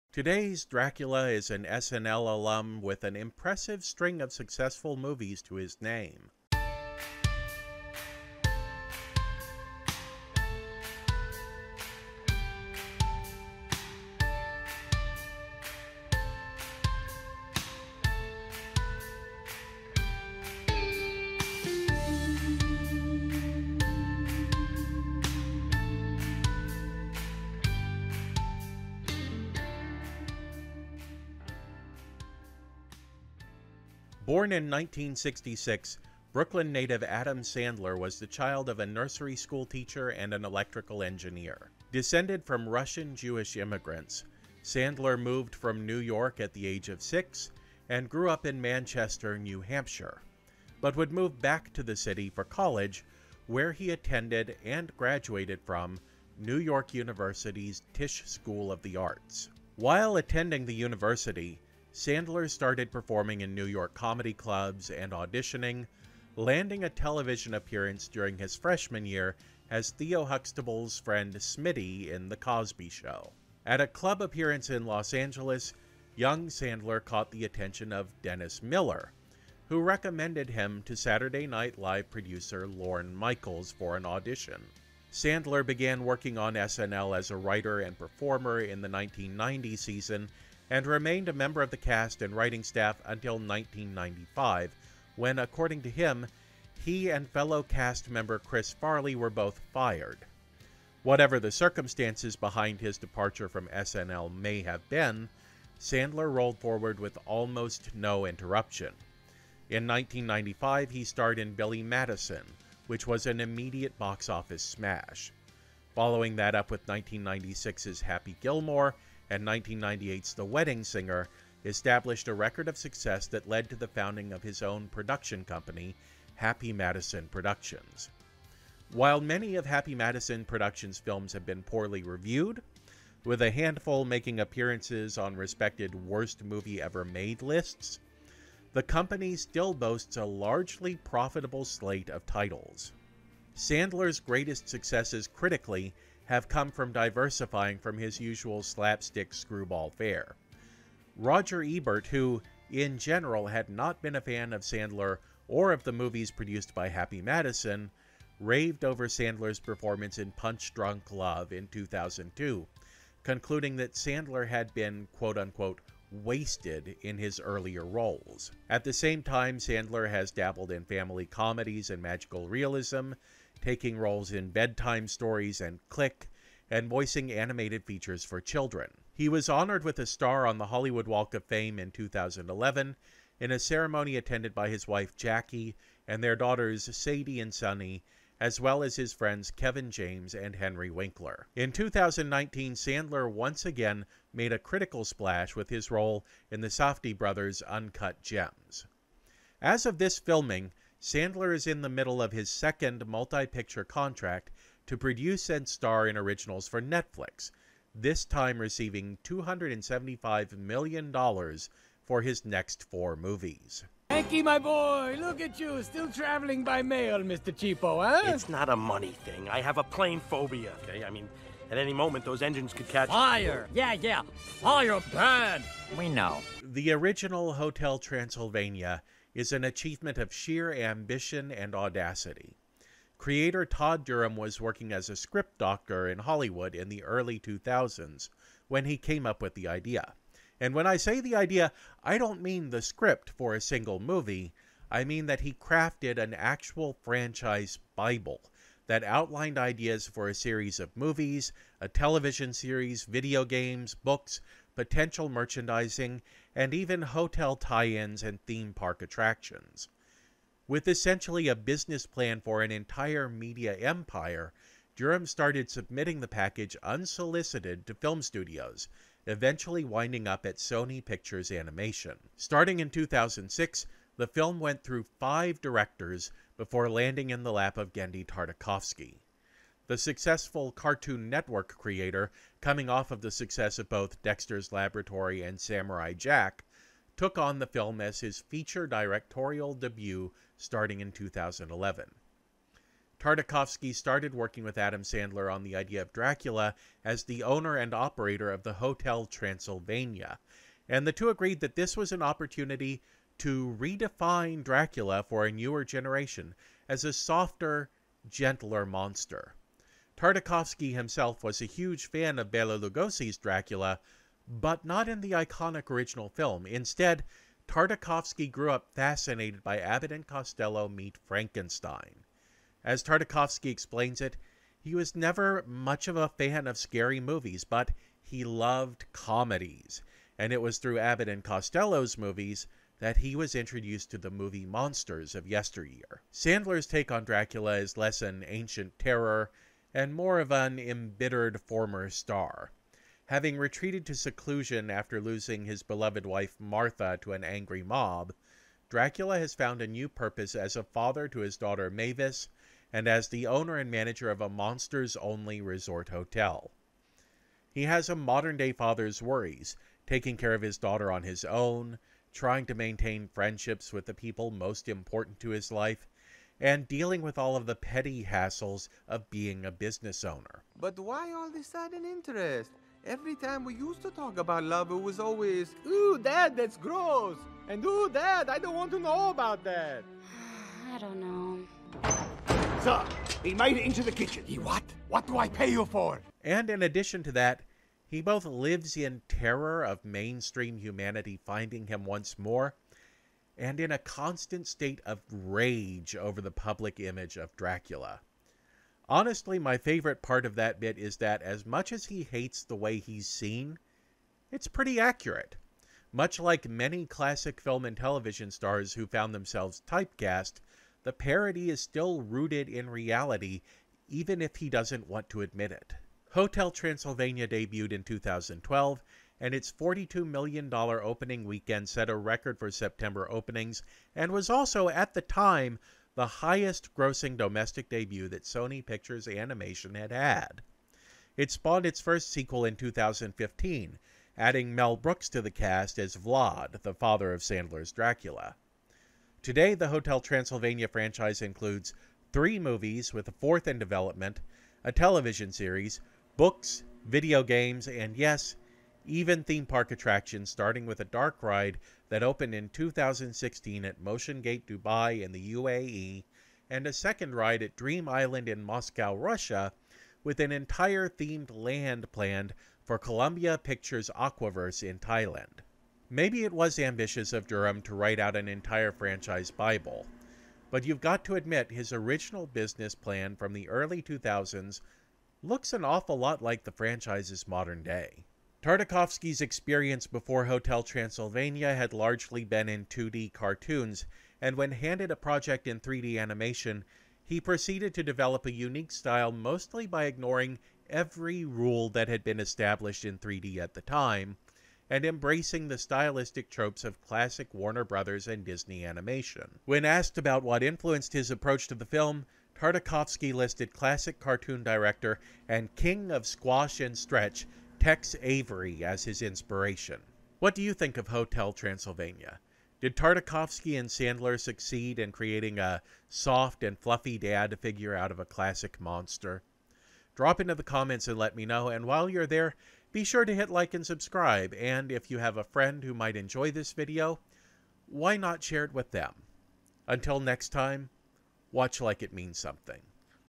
The Film Optimist's Countdown of Draculas #2 - Adam Sandler in "Hotel Transylvania" (2012)
The Film Optimist is made possible by viewers like you!